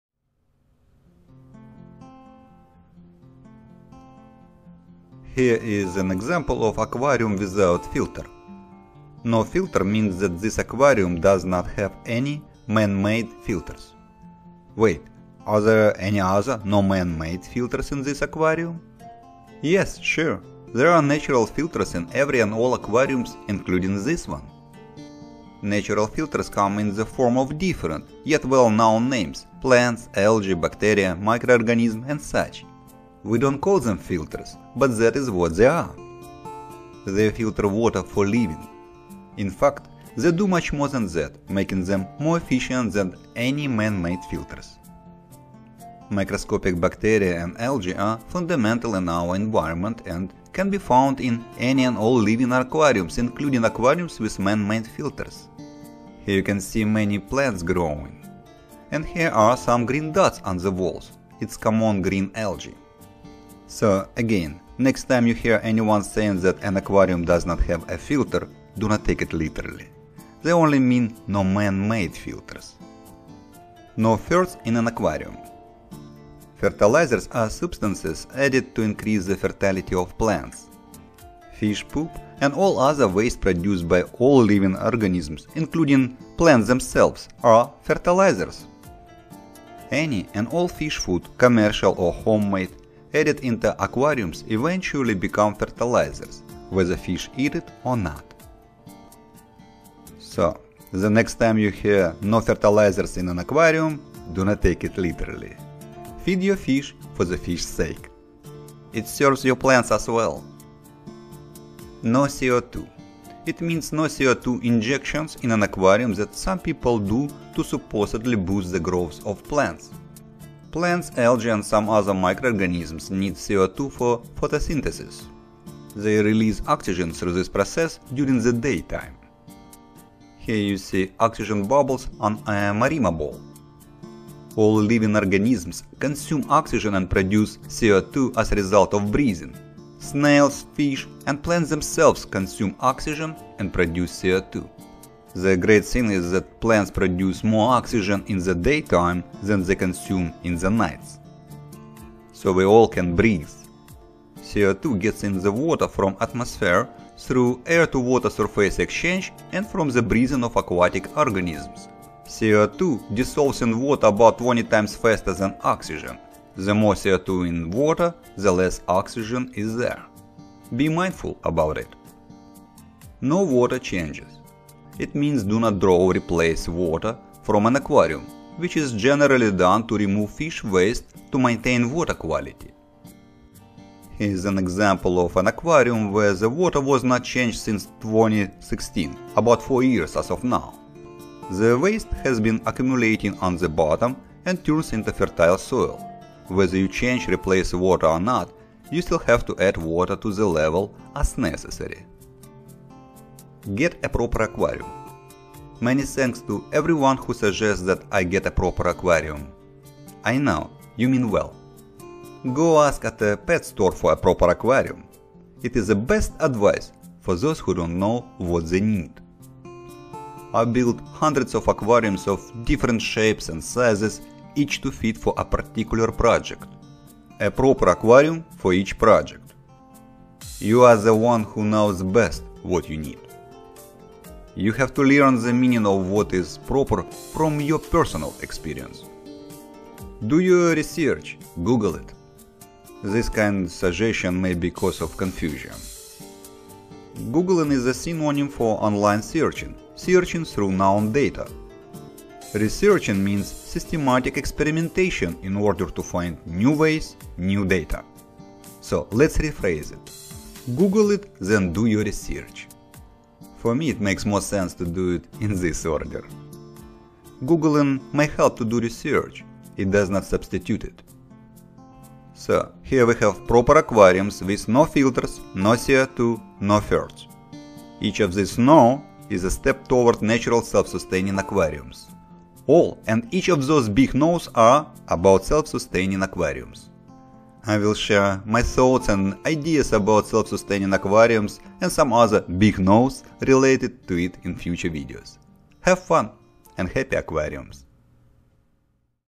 NO Filter, NO CO2, NO Ferts Aquarium - What is it all about
00:00 Title
00:05 Here is an example of aquarium without filter.
No Filter means that this aquarium does not have any man-made filters.
Wait, are there any other, no man-made filters in this aquarium?
Yes. Sure.
00:22 There are natural filters in every and all aquariums, including this one.
Natural filters come in the form of different, yet well known names - plants, algae, bacteria, microorganisms and such.
We do not call them filters, but that is what they are!
They filter water for a living.
00:40 In fact, they do much more than that, making them more efficient than any man-made filters.
Microscopic bacteria and algae are fundamental in our environment and can be found in any and all living aquariums, including aquariums with man-made filters.
Here you can see many plants growing.
And here are some green dots on the walls - it's common green algae.
01:04 So again, next time you hear anyone saying that an aquarium does not have a filter, do not take it literally - they only mean no man-made filters.
01:14 No Ferts in an aquarium.
Fertilizers are substances added to increase the fertility of plants.
Fish poop and all other waste produced by all living organisms, including plants themselves, are fertilizers.
Any and all fish food, commercial or homemade, added into aquariums eventually becomes fertilizers, whether fish eat it or not.
So, the next time you hear No Fertilizers in an aquarium - do not take it literally!
Feed your fish for the fish’s sake :)
It serves your plants as well.
01:52 NO CO2
It means no CO2 injections in an aquarium that some people do to supposedly boost the growth of plants.
Plants, algae, and some other microorganisms need CO2 for photosynthesis.
They release Oxygen through this process during the daytime.
Here you see oxygen bubbles on a Marimo ball.
All living organisms consume Oxygen and produce CO2 as result of breathing.
Snails, fish and ... plants themselves consume Oxygen and produce CO2.
The great thing is that plants produce more Oxygen in the daytime than they consume in the nights.
So we all can breath :)
CO2 gets in the water from atmosphere through air to water surface exchange and from the breathing of aquatic organisms.
CO2 dissolves in water about 20 times faster than Oxygen.
The more CO2 is in water, the less Oxygen is there.
Be mindful about it.
02:53 No Water Changes.
It means do not draw (replace) water from an aquarium - which is generally done to remove fish waste to maintain water quality.
Here is an example of an aquarium where the water was not changed since 2016, about 4 years as of now
The waste has been accumulating on the bottom and turns into fertile soil.
Whether you change (replace) water or not, you still have to add water to the level as necessary.
03:26 Get a Proper Aquarium.
Many thanks to everyone who suggest that I get a proper aquarium.
I know you mean well!
Go ask at a pet store for a proper aquarium!
It is the best advice...for those who don't know what they need.
I build hundreds of aquariums of different shapes and sizes, each to fit for a particular project.
You are the one who knows best what you need.
You have to learn the meaning of what is proper from your personal experience.
04:00 Do Your Research - "Google" it.
This kind suggestion may be the cause of confusion.
Googling is a synonym for online searching.
Searching through known data.
Researching means systematic experimentation in order to find new ways, new data.
So, let's rephrase it.
"Google" it then do your research.
For me it makes more sense to do it in this order ;)
Googling may help to do research.
It does not substitute it!
04:33 So, here we have Proper aquariums with NO Filters, NO CO2, NO Ferts.
Each of this NO is a step toward Natural Self-Sustaining aquariums.
All and each of those big NOs are about Self Sustaining aquariums.
I will share my thoughts and ideas about self-sustaining aquariums and some other Big NOs related to it in future videos
Have fun and happy aquariums :)